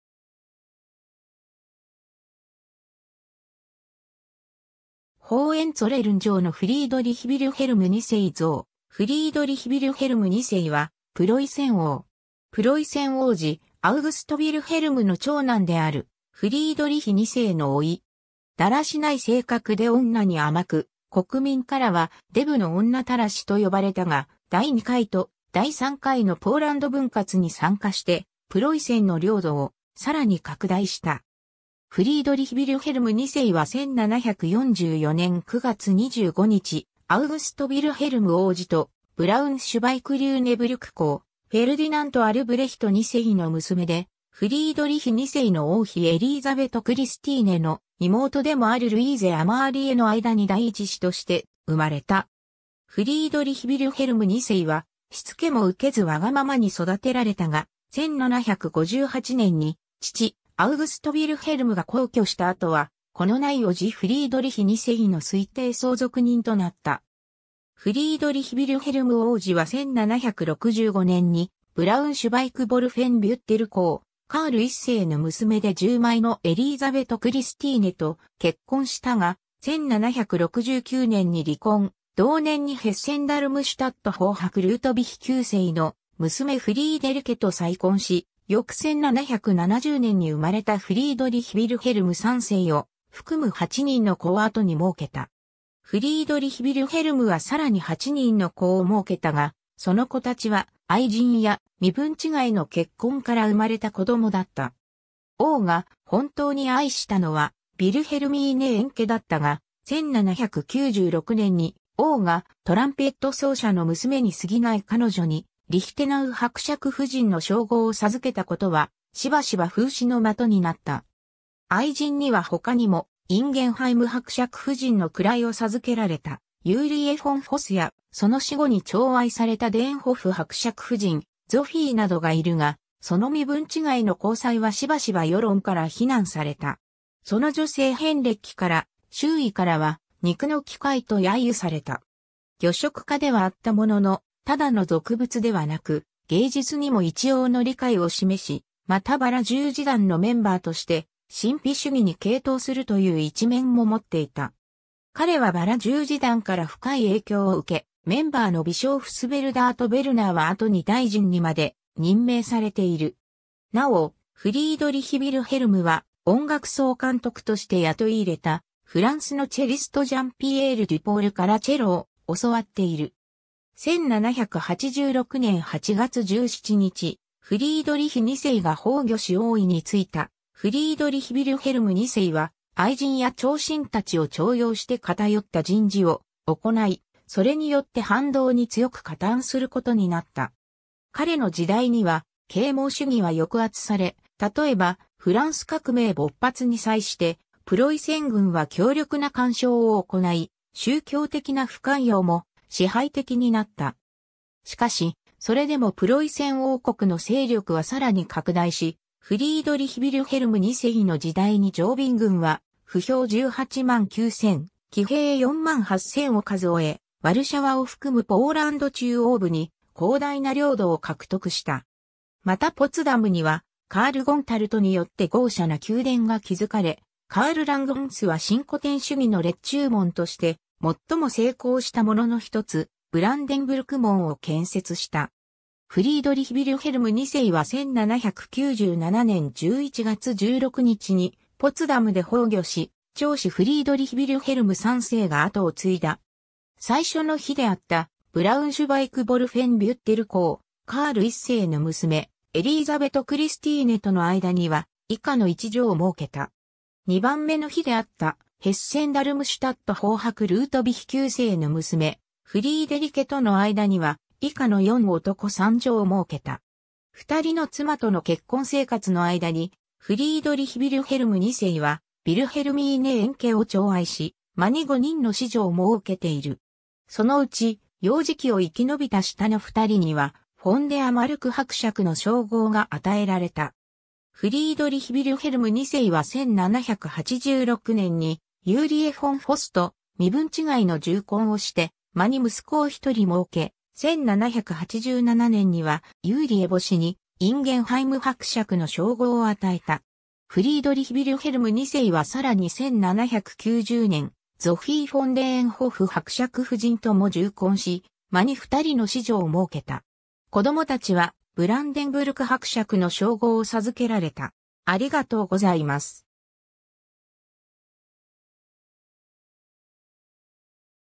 フリードリヒ・ヴィルヘルム2世 (プロイセン王)
ホーエンツォレルン城のフリードリヒ・ヴィルヘルム2世像 フリードリヒ・ヴィルヘルム2世（Friedrich Wilhelm II., 1744年9月25日 - 1797年11月16日）は、プロイセン王（在位：1786年8月17日 - 1797年11月16日）。
プロイセン王子アウグスト・ヴィルヘルムの長男であるフリードリヒ2世の甥。
だらしない性格で女に甘く、国民からは「でぶの女たらし」（der Vielgeliebte ("the much loved") und der dicke Lüderjahn ("the fat scallywag")）と呼ばれたが、第2回と第3回のポーランド分割に参加してプロイセンの領土をさらに拡大した。
 フリードリヒ・ヴィルヘルム2世は1744年9月25日、アウグスト・ヴィルヘルム王子と、ブラウンシュヴァイク＝リューネブルク公フェルディナント・アルブレヒト2世の娘でフリードリヒ2世の王妃エリーザベト・クリスティーネの妹でもあるルイーゼ・アマーリエ（1722年 - 1780年）の間に第一子として生まれた。
フリードリヒ･ヴィルヘルム2世はしつけも受けずわがままに育てられたが、1758年に父アウグスト・ヴィルヘルムが薨去した後は子のない伯父フリードリヒ2世の推定相続人となった。
 フリードリヒ・ヴィルヘルム王子は1765年にブラウンシュヴァイク＝ヴォルフェンビュッテル公カール1世の娘で従妹のエリーザベト・クリスティーネと結婚したが、1769年に離婚、同年にヘッセン＝ダルムシュタット方伯ルートヴィヒ9世の娘フリーデリケ（1751年 – 1805年）と再婚し、翌1770年に生まれたフリードリヒ・ヴィルヘルム3世を含む8人の子を後にもうけた。
 フリードリヒ・ヴィルヘルムはさらに8人の子をもうけたが、その子達は愛人や身分違いの結婚から生まれた子供だった。
王が本当に愛したのはヴィルヘルミーネ・エンケだったが、1796年に王がトランペット奏者の娘に過ぎない彼女にリヒテナウ伯爵夫人の称号を授けたことは、しばしば風刺の的になった。
愛人には他にも、インゲンハイム伯爵夫人の位を授けられたユーリエ・フォン・フォスや、その死後に寵愛されたデーンホフ伯爵夫人ゾフィーなどがいるが、その身分違いの交際はしばしば世論から非難された。
その女性遍歴から周囲からは「肉の機械」と揶揄された。
 漁色家ではあったものの、ただの俗物ではなく、芸術にも一応の理解を示し、また薔薇十字団のメンバーとして神秘主義に傾倒するという一面も持っていた。
彼は薔薇十字団から深い影響を受け、メンバーのビショーフスヴェルダーとヴェルナーは後に大臣にまで任命されている。
なお、フリードリヒ・ヴィルヘルムは音楽総監督として雇い入れたフランスのチェリストジャン・ピエール・デュポールからチェロを教わっている。
 1786年8月17日、フリードリヒ2世が崩御し王位に就いたフリードリヒ・ヴィルヘルム2世は、愛人や寵臣たちを重用して偏った人事を行い、それによって反動に強く加担することになった。
彼の時代には啓蒙主義は抑圧され、たとえばフランス革命勃発に際してプロイセン軍は強力な干渉を行い、宗教的な不寛容も支配的になった。
 しかしそれでもプロイセン王国の勢力はさらに拡大し、フリードリヒ・ヴィルヘルム2世の時代に常備軍は歩兵18万9千・騎兵4万8千を数え、ワルシャワを含むポーランド中央部に広大な領土を獲得した。
またポツダムにはカール・ゴンタルトによって豪奢な宮殿が築かれ、カール・ラングハンスは新古典主義の列柱門として最も成功したものの1つ、ブランデンブルク門を建設した。
 フリードリヒ･ヴィルヘルム2世は1797年11月16日にポツダムで崩御し、長子フリードリヒ・ヴィルヘルム3世が後を継いだ。
 最初の妃であったブラウンシュヴァイク＝ヴォルフェンビュッテル公カール1世の娘エリーザベト・クリスティーネとの間には、以下の1女をもうけた。
 2番目の妃であったヘッセン＝ダルムシュタット方伯ルートヴィヒ9世の娘フリーデリケとの間には、以下の4男3女をもうけた。
 二人の妻との結婚生活の間に、フリードリヒ・ヴィルヘルム2世はヴィルヘルミーネ・エンケ（のちにリヒテナ...